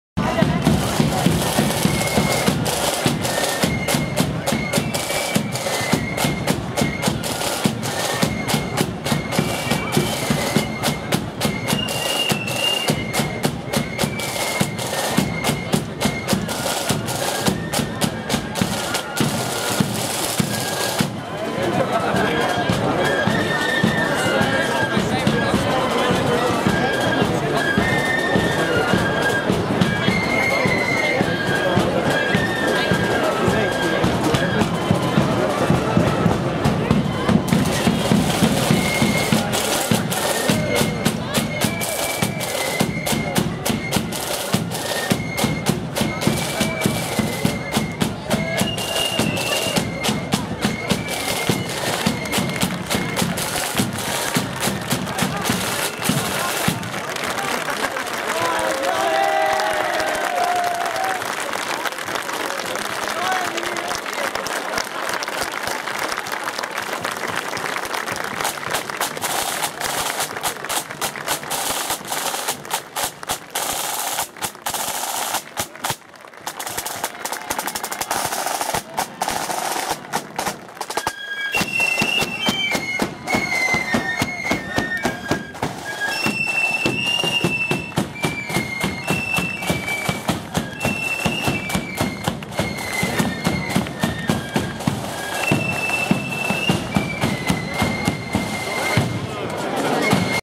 Dunamoney FB @ Ballymaconnelly Sons Of Conquerors FB Parade 2012
Congratulations to Ballymaconnelly Sons Of Conquerors For The Way In Which They Marshalled Their Parade In Rasharkin Friday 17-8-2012.
Once again they showed Respect & Dignity to those within the community.
Both Protestants & Catholics.
Remember David, sorry Daithi.
Sticks And Stones Will Break Your Bones But Flutes Will Never Harm You.

By the way Daithi too.
Its not the Protestant Unionist Loyalist Communties Fault That Sectarian Sinn Fein are under Pressure From Other Republican Gangs.